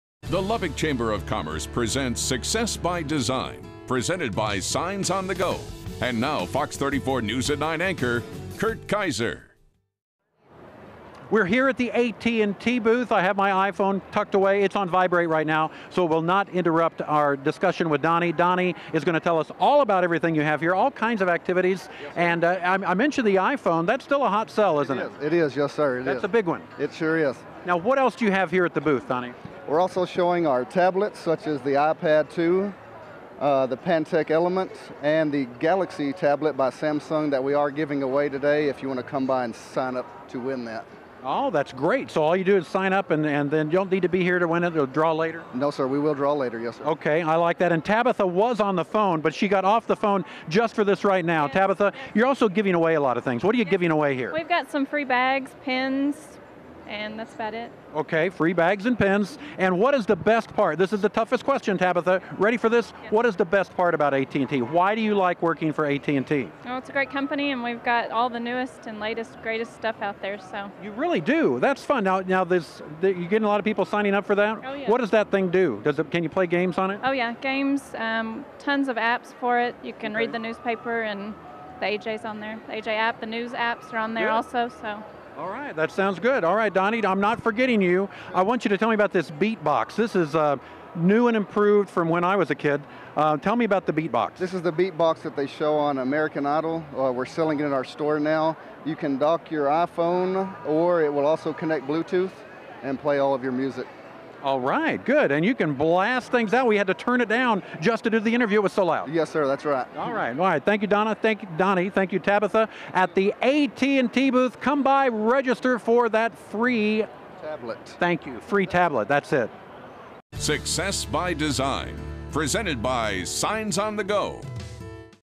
AT&T Business Expo Interview
During the 2012 Business Expo - Success by Design, Fox 34 interviewed sponsors of the event, including AT&T!